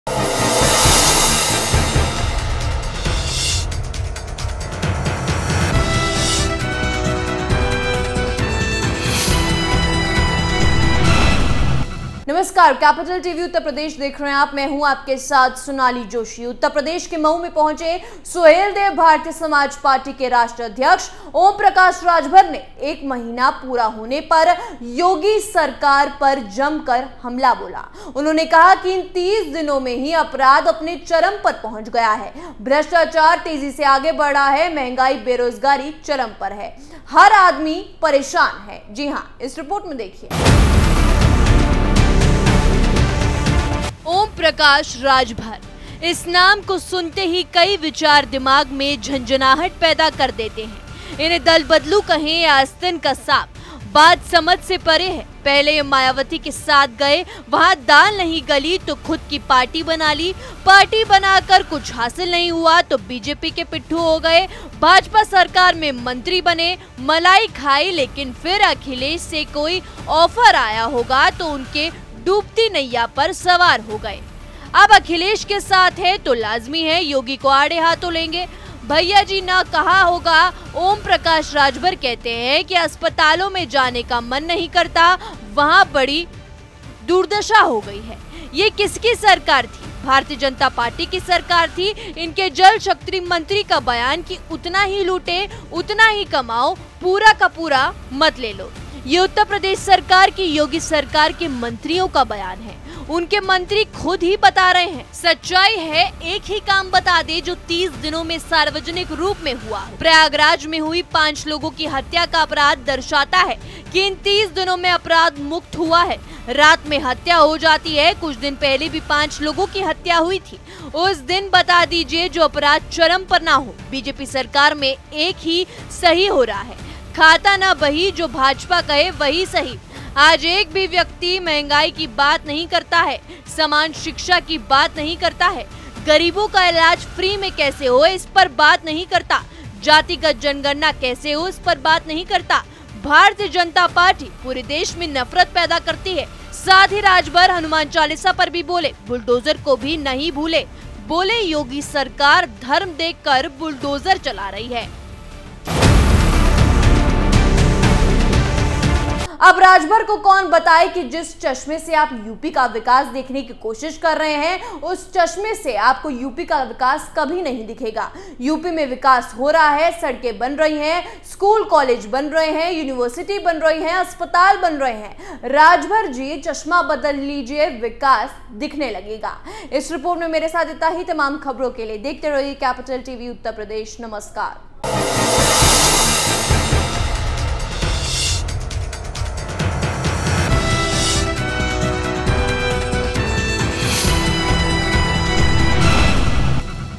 OP Rajbhar Kept The Report Card Of Yogi Government | Capital TV Uttar Pradesh
Om Prakash Rajbhar, the national president of Suheldev Bharatiya Samaj Party, who reached Mau in Uttar Pradesh, attacked the Yogi government fiercely on the completion of 1 month. He said that crime has reached its peak in these 30 days. Corruption is increasing rapidly and inflation and unemployment is at their peak every man is troubled.